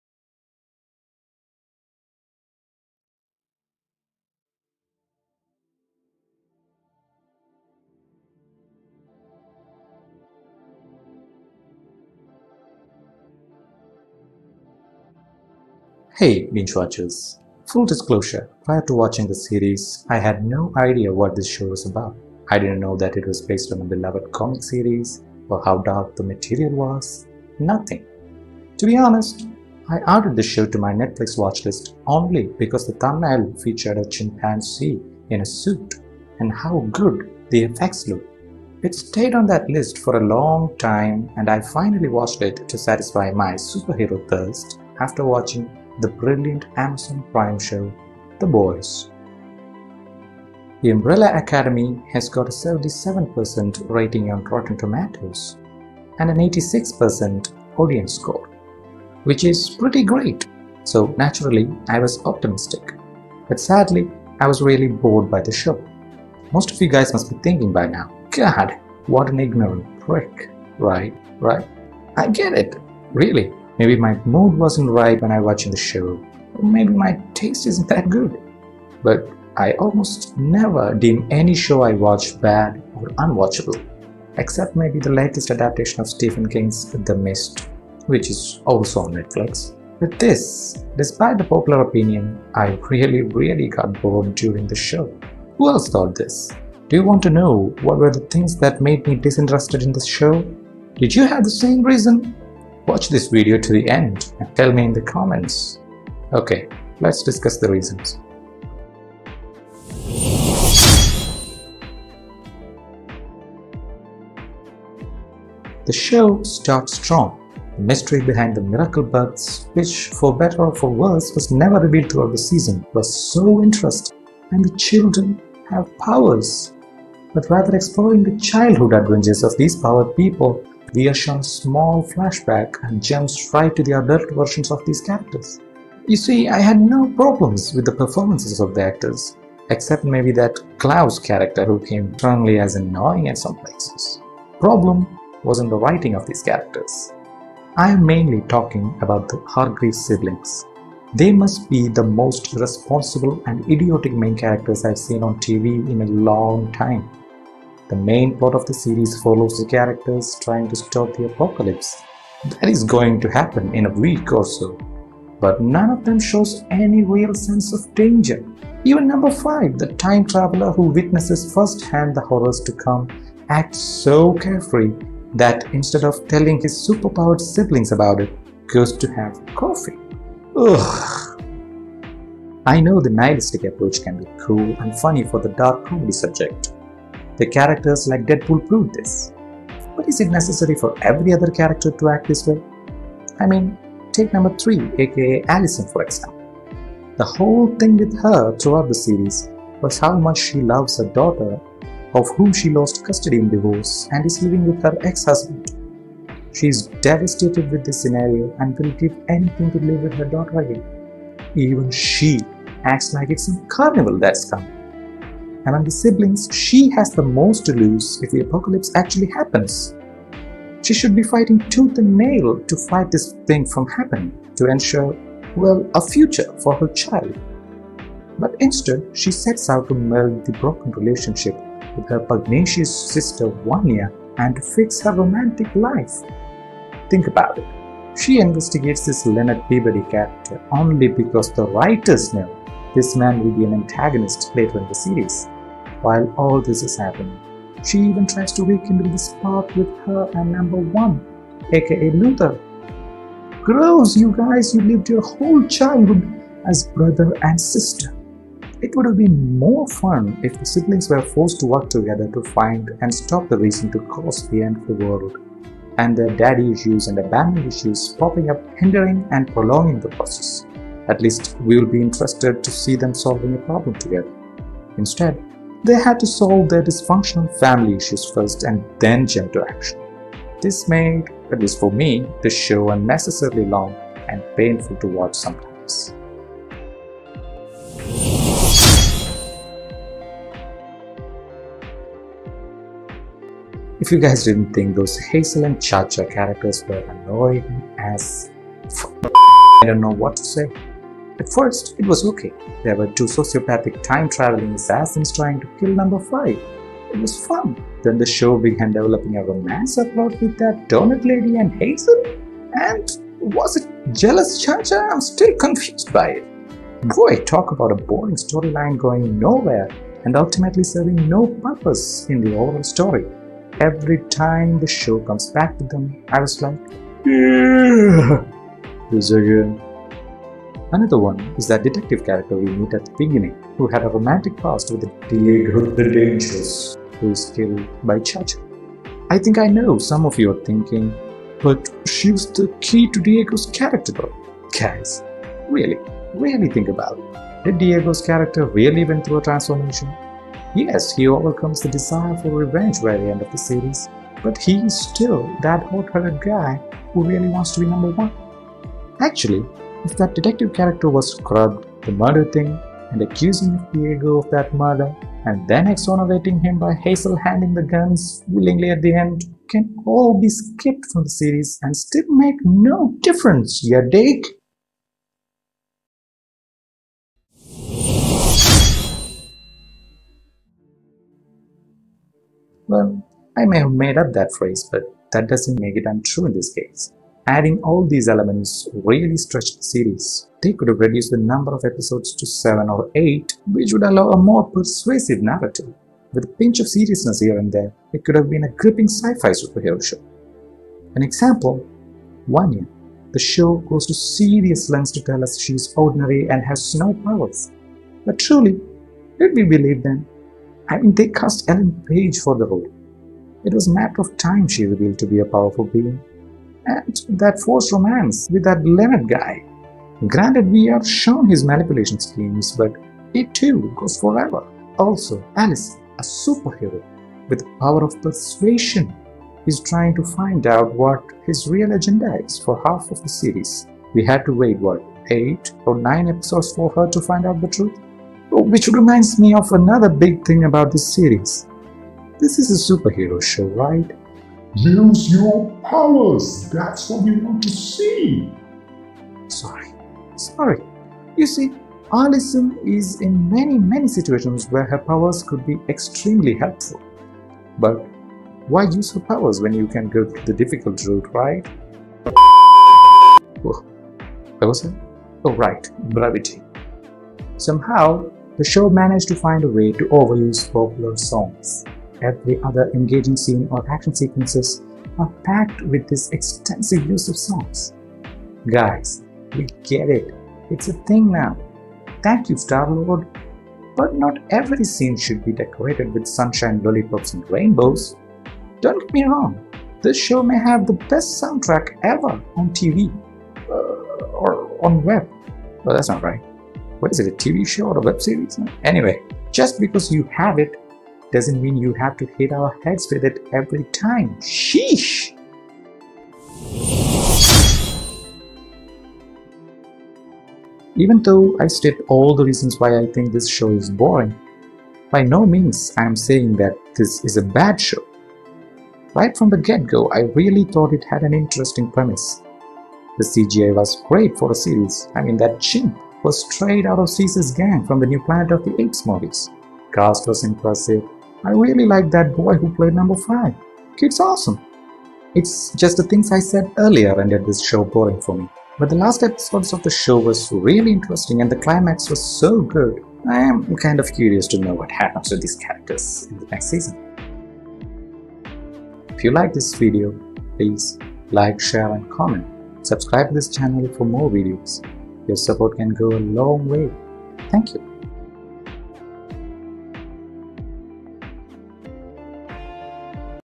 Why 'The Umbrella Academy' is BORING? - Potato rant
Can we dislike a universally loved TV show? CluelessBingewatcher discusses the many reasons to why NETFLIX's TheUmbrellaAcademy is boring, despite the generally well reception.

The Umbrella Academy is a weird but boring superhero Web series based on the comic books of the same name written by Gerard Way of 'My Chemical Romance'.